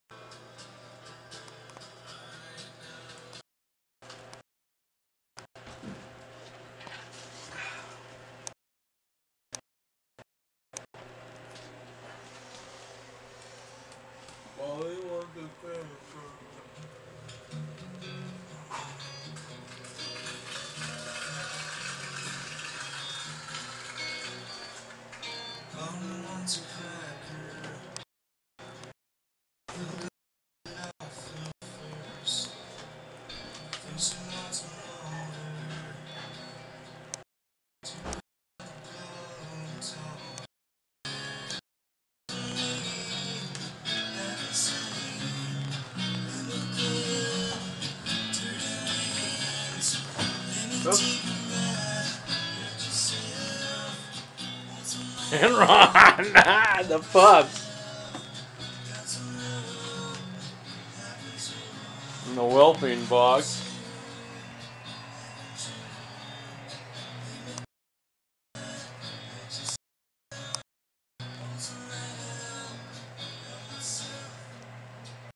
Frenchie and Pups 003_NEW.avi
Frenchie & No No's Pups they only replaced themselves. But aren.'t they cute They are only few days old on this video.